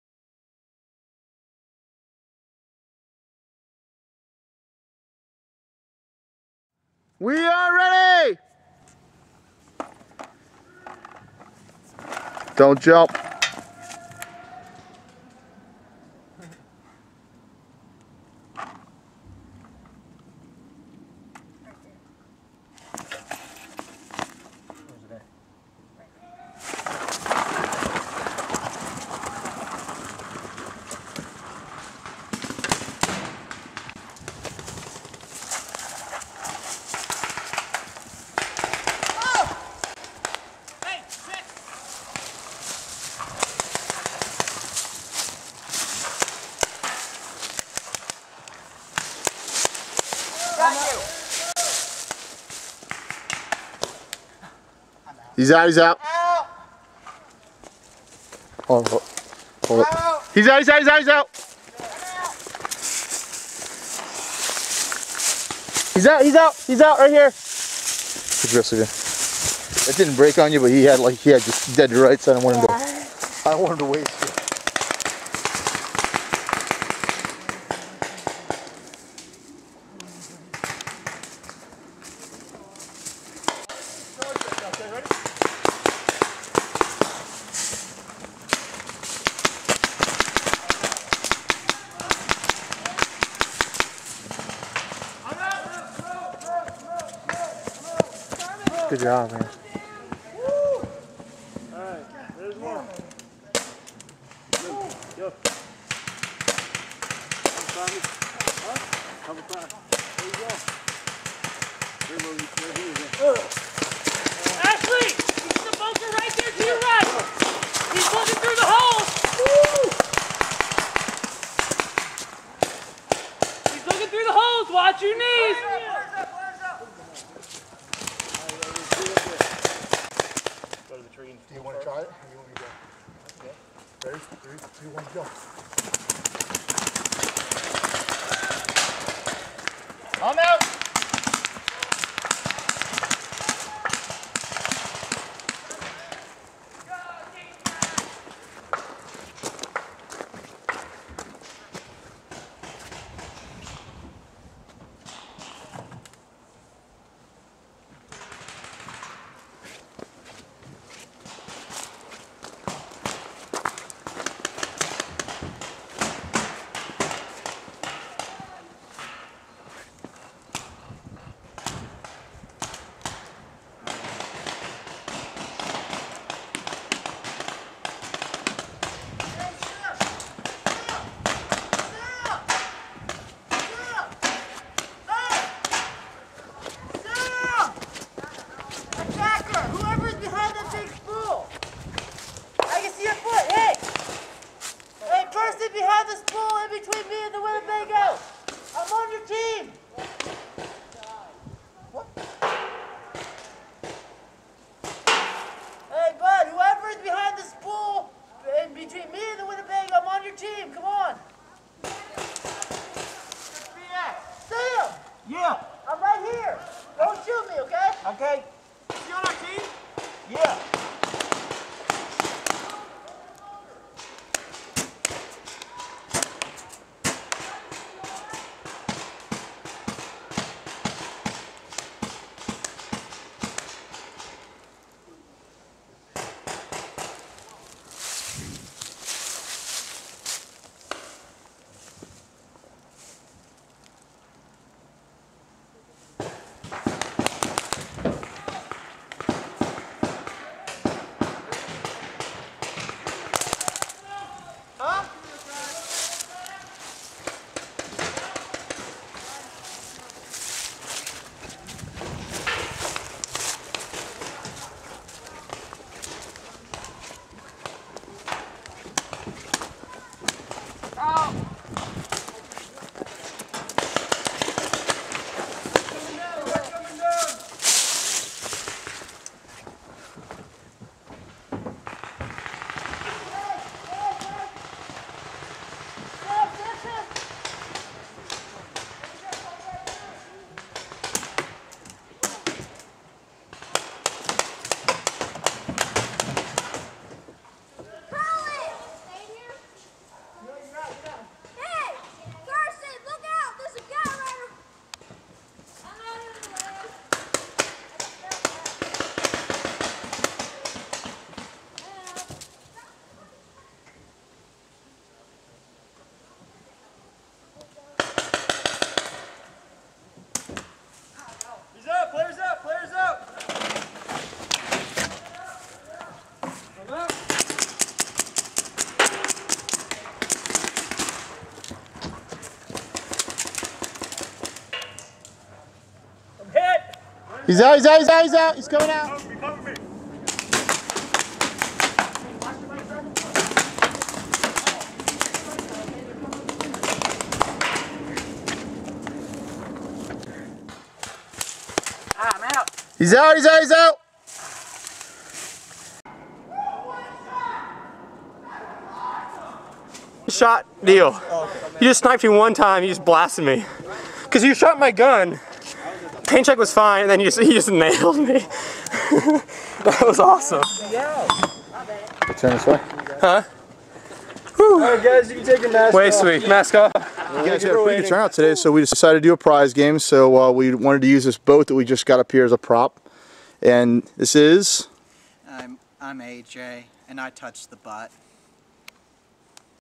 LU & UVA Paintball
Students from Liberty University and UVA play paintball at Big Pearl Paintball on an open day.